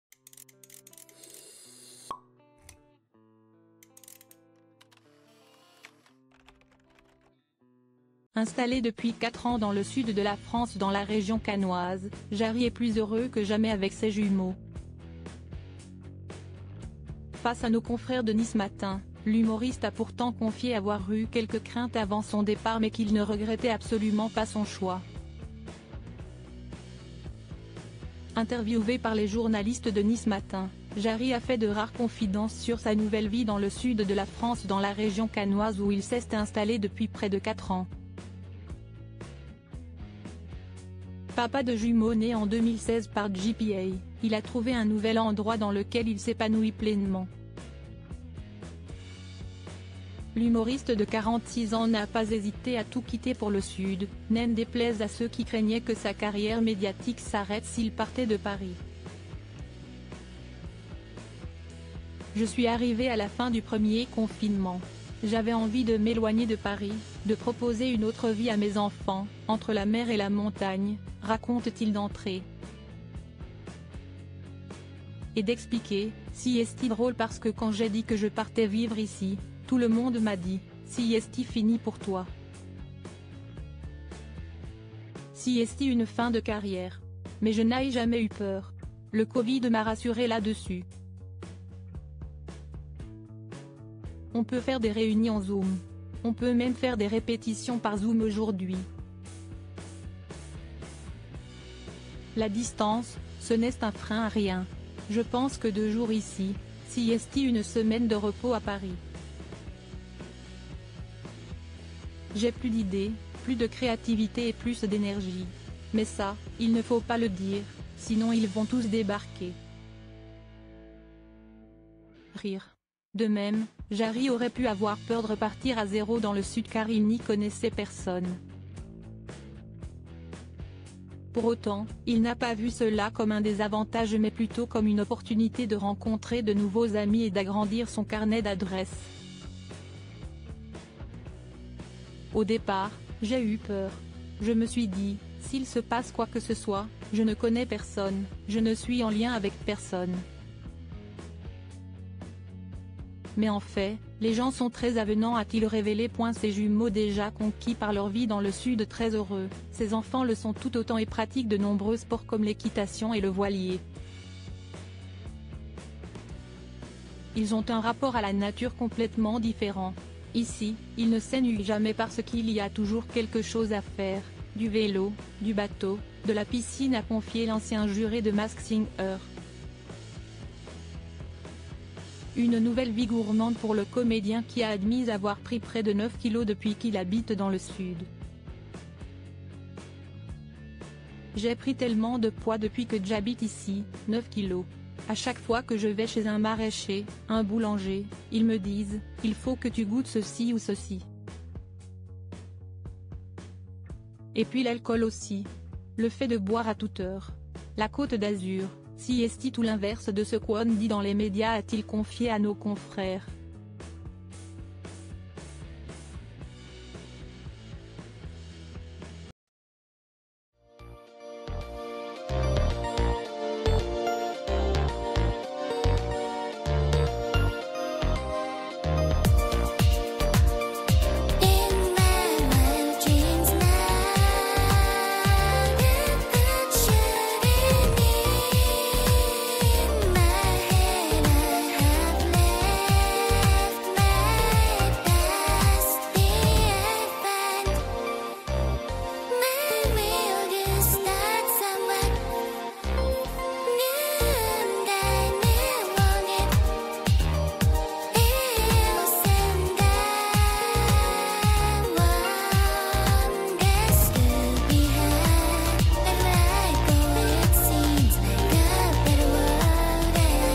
Jarry et ses jumeaux heureux d'avoir quitté Paris malgré une mise en garde précise de ses proches, "
Jarry et ses jumeaux heureux d'avoir quitté Paris malgré une mise en garde précise de ses proches, "tout le monde m'a dit..."